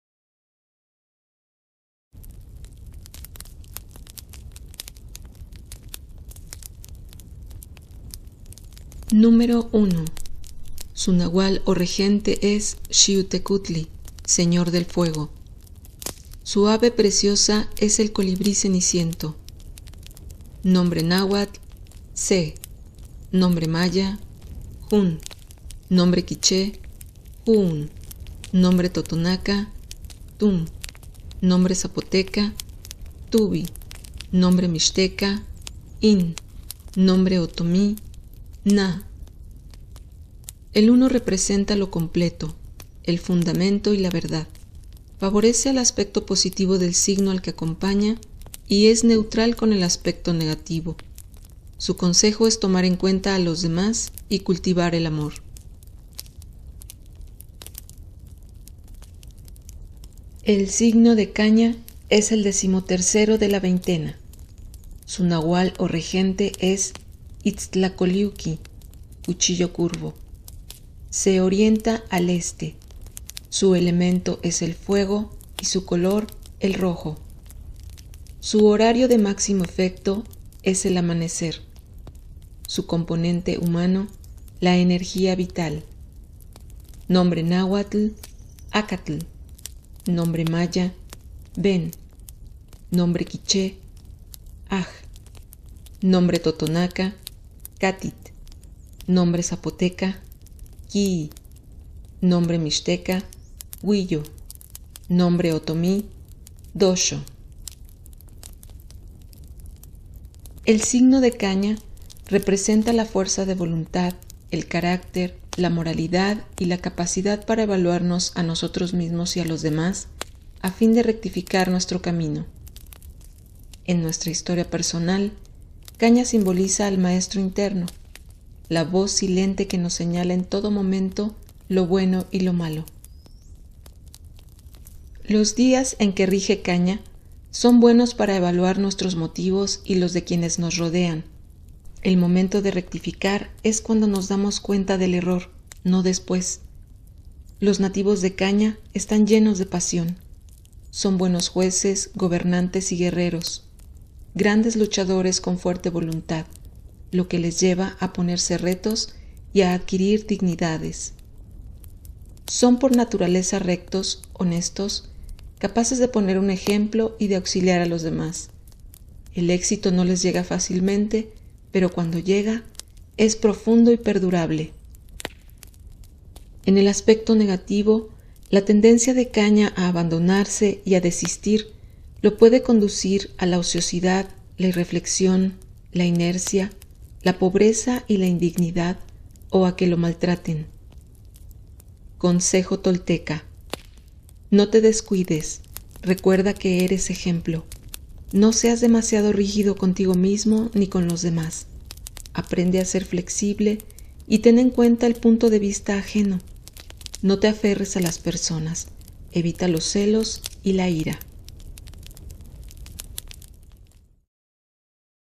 El tonal de hoy es "1 Caña - Se Akatl" 30/05/24
Día 1 Caña
20 de Toskatl
Año 8 Ocelote
50 del 19 Atado
5146 del Quinto Sol

30 de mayo de 2024.